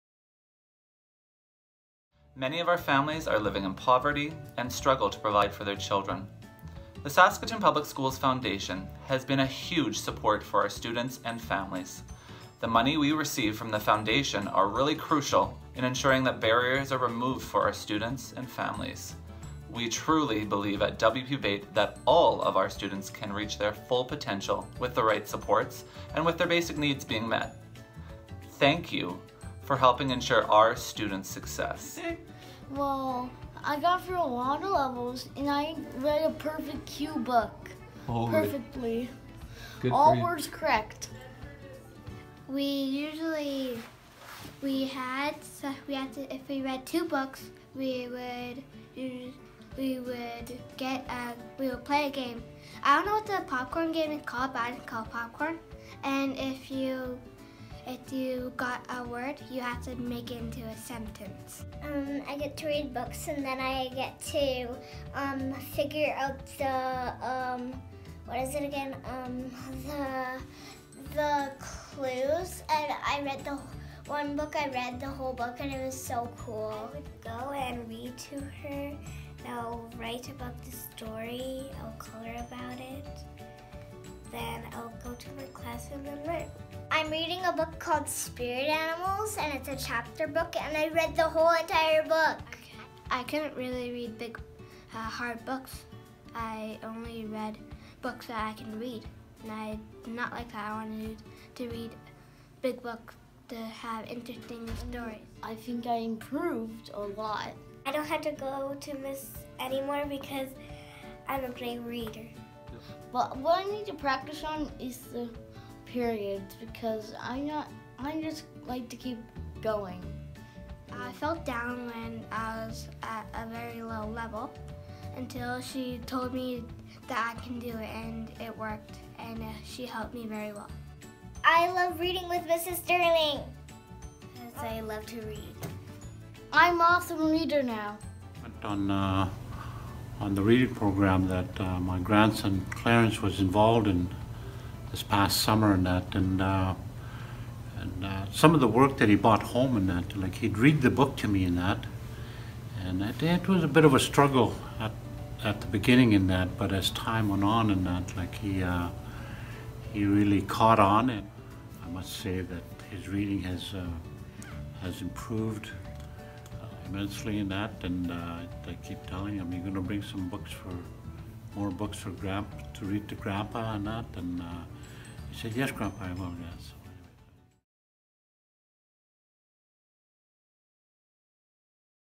W.P. Bate Literacy Students
Principal Dean Swan and students from W.P. Bate School talk about the importance of SPSF's literacy supports.